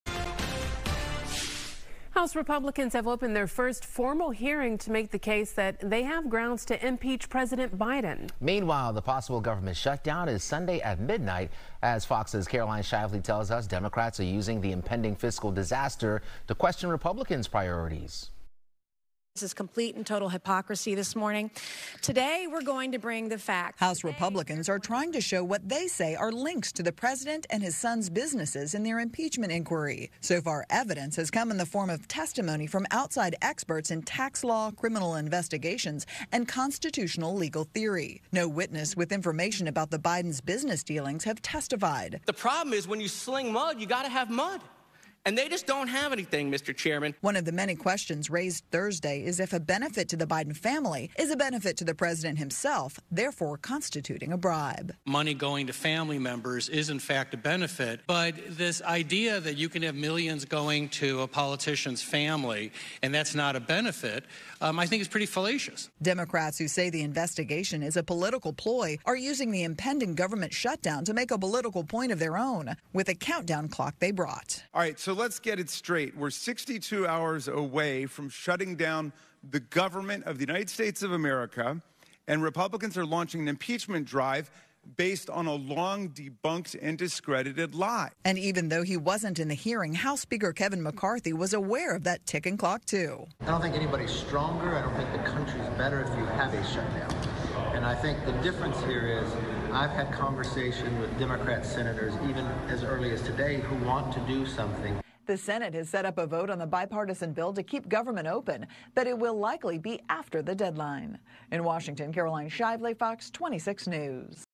House Republicans seek grounds to remove Biden
House Republicans hold their first formal hearing to argue grounds for impeaching President Biden. At the same time, Democrats use the looming government shutdown as a platform to question Republican priorities, reports FOX's Caroline Shively.